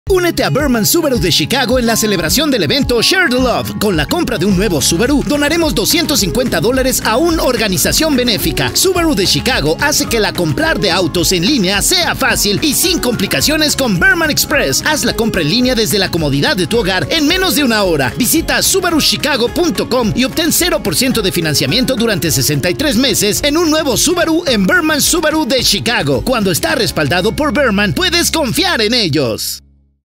Evento Share the Love de Subaru | Berman Subaru de Chicago
Con el Evento Share The Love de Subaru, obtén un financiamiento del 0% durante 63 meses para un nuevo Subaru y donaremos $250 a una organización benéfica local. Ahora hasta el 30 de noviembre de 2020